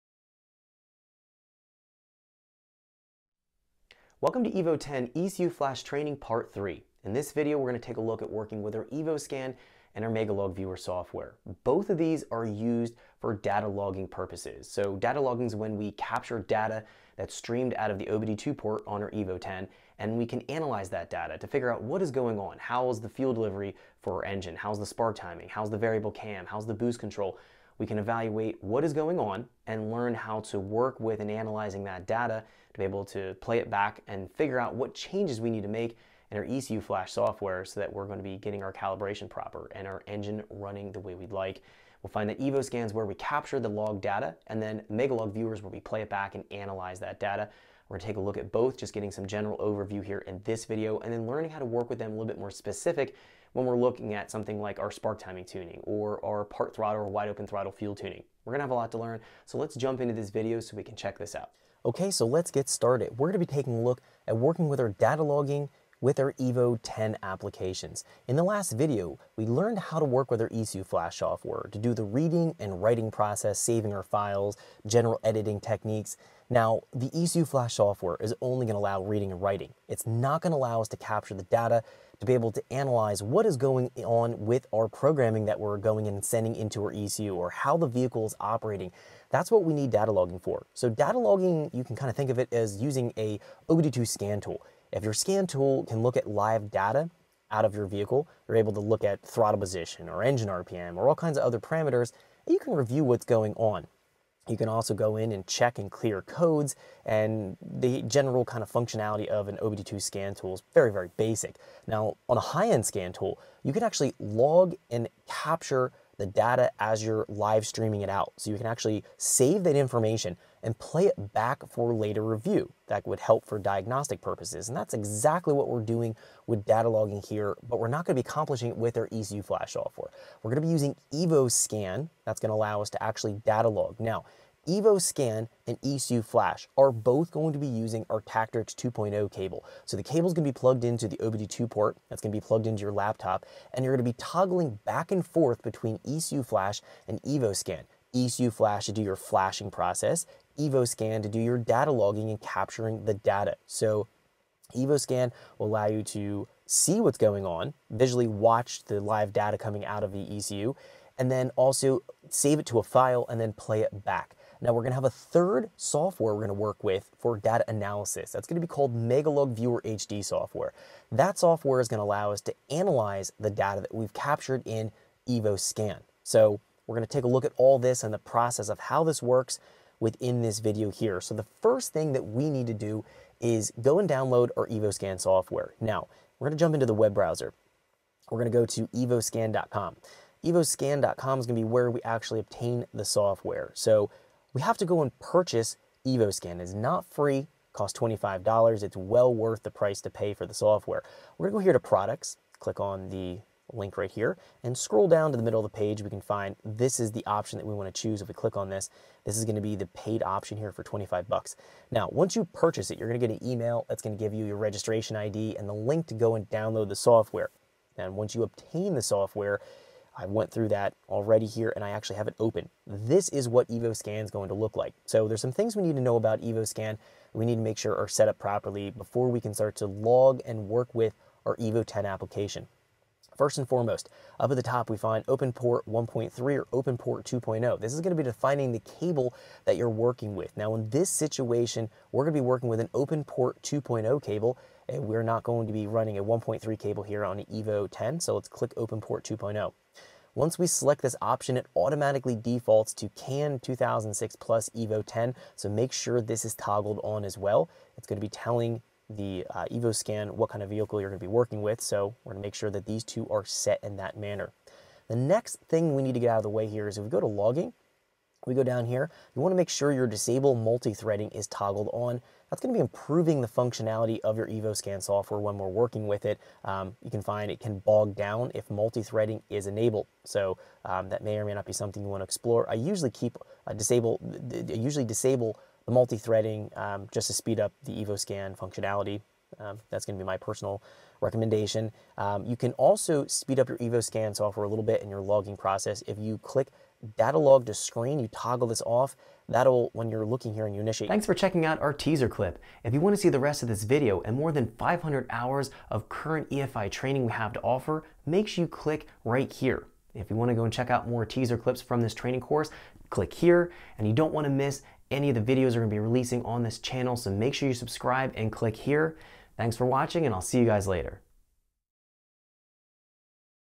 Evo X Ecu Flash Training Course Part 3: EvoScan & Megalog Viewer HD | Evans Performance Academy
Evo X Ecu Flash Training Course Part 3: Evo Scan & Megalog Viewer Software Overview walks you a basic over view of how the Evo Scan and Megalog Viewer works to begin our training course.